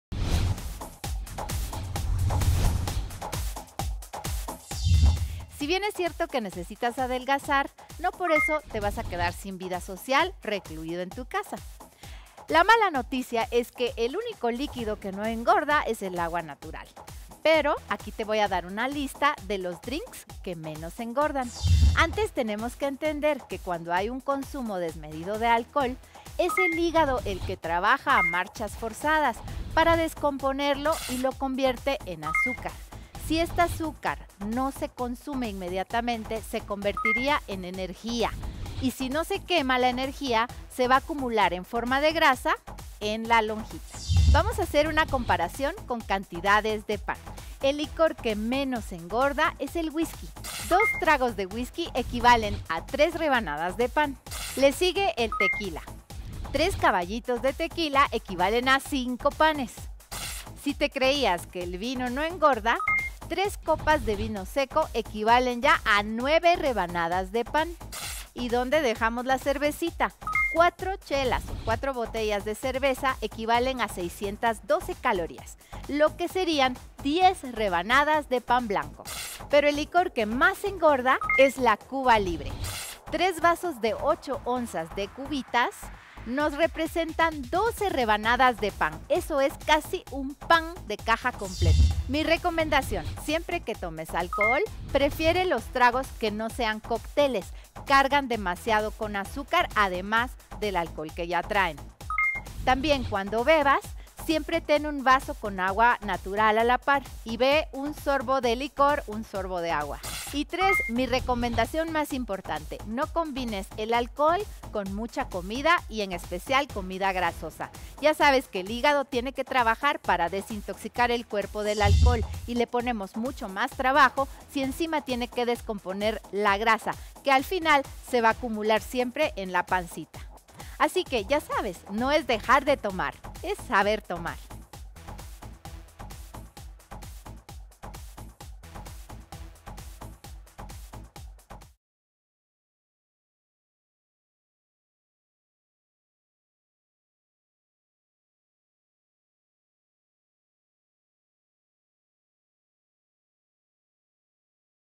¿Cuál es el alcohol que engorda menos? @RBKTD te lo explica en este video
Rebeca Rubio te explñica en sus RebeTips cuál es el alcohol que engorda menos.
Esto es